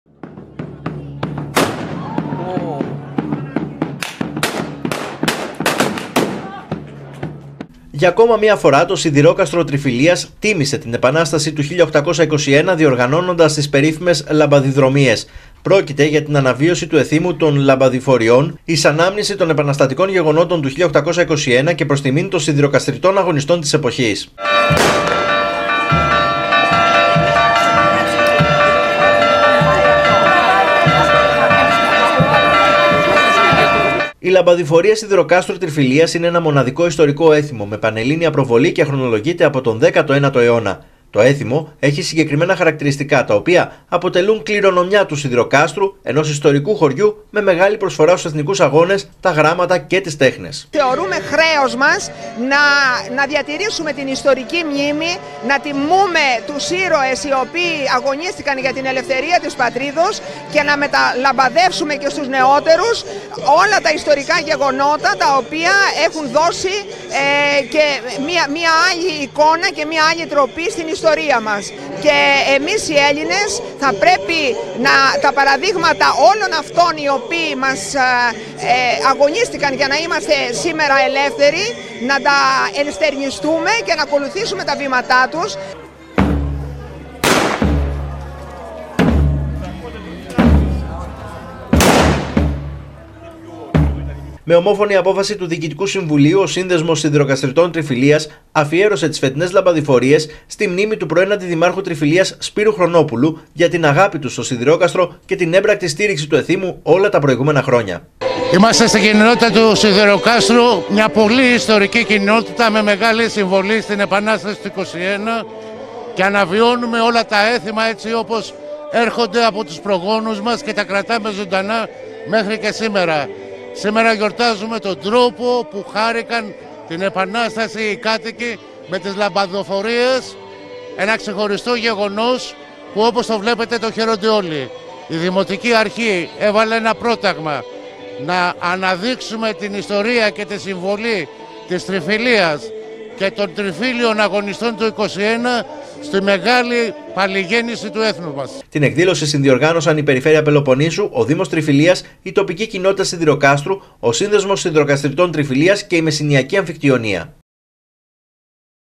ΜΕΣΣΗΝΙΑ | Αναβίωσε το έθιμο των λαμπαδηφοριών στο Σιδηρόκαστρο Μεσσηνίας
Το έθιμο των λαμπαδηφοριών προσέλκυσε πολύ κόσμο, για μια ακόμα χρονιά στο Σιδηρόκαστρο Μεσσηνίας. Πρόκειται για ένα έθιμο που κρατάει από τον 19ο αιώνα και έχει ως σκοπό να τιμήσει τους αγωνιστές του 1821